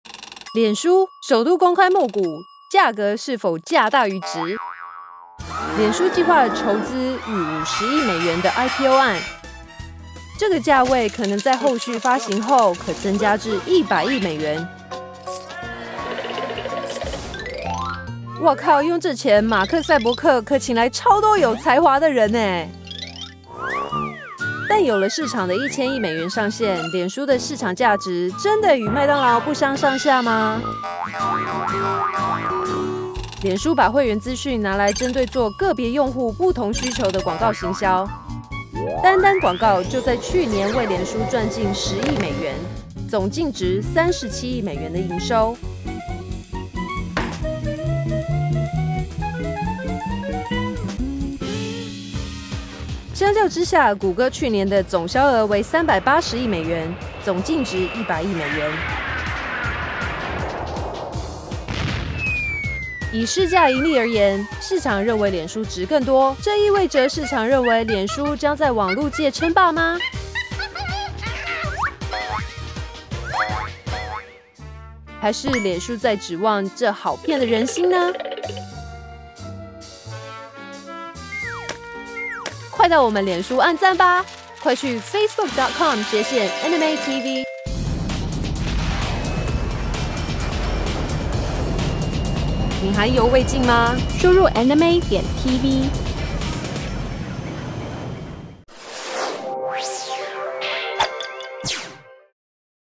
Facebook IPO: is Mark Zuckerberg's company overvalued?
Facebook's IPO is planned to raise US$5 billion; that could value the company at US$100 billion. The eight-year-old company founded by Mark Zuckerberg could raise as much as US$10 billion with follow-on offerings.

A market cap of US$100 billion would make Facebook one of the most valuable companies in the world, on par with McDonald's.

The company sells highly targeted ads based on user-provided information. This helped Facebook earn US$1 billion last year on US$3.7 billion in revenue. By comparison, Google pulled in US$38 billion in sales and US$10 billion in earnings.

But on a price-to-sales basis, the market thinks Facebook is worth more. Does this mean the market thinks Facebook will win the internet?

Analysts expect Facebook's advertising revenue will grow just 52% to $5.8 billion this year and only 21% to $7 billion in 2013. That is still far behind Google.

What's more, Facebook is increasingly popular on mobile devices but doesn't display ads on phones and its capability to do so remains unproven.

Sales from virtual goods in games aren't much help either. Virtual goods produced just 12% of Facebook's revenue last year.